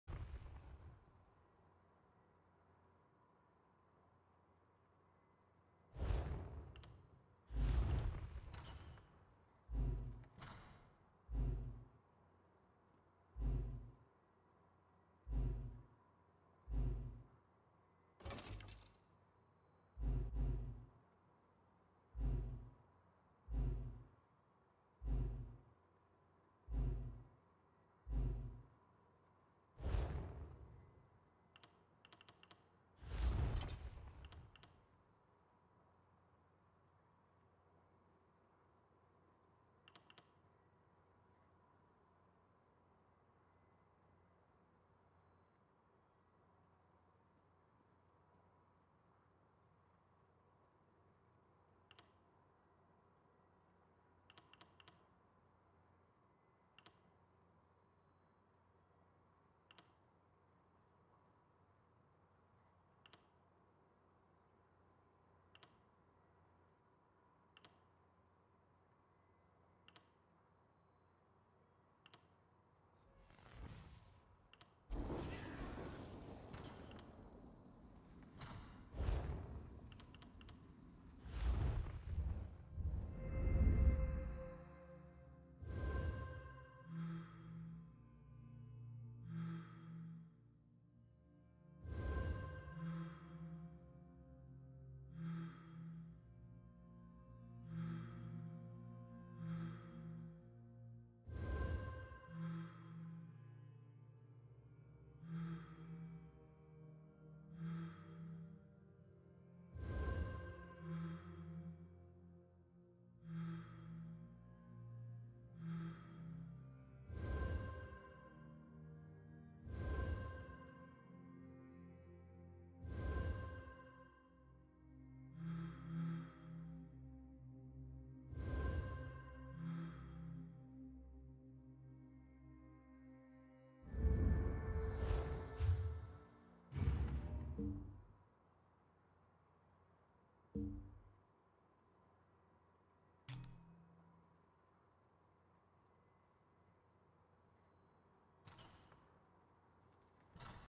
2H/Bow Stamina Sorcerer Build for VMA
Let me know if i forgot anything. Just a friendly reminder to everyone that gear, and your cp layout will only get you so far in VMA. If you want to get good scores in the instance it's about;knowing the character you're playing, knowing spawn points of the instance, knowing mechanics of the instance, and having a little bit of rng float your way sometimes. So, as the old saying goes, practice makes perfect :).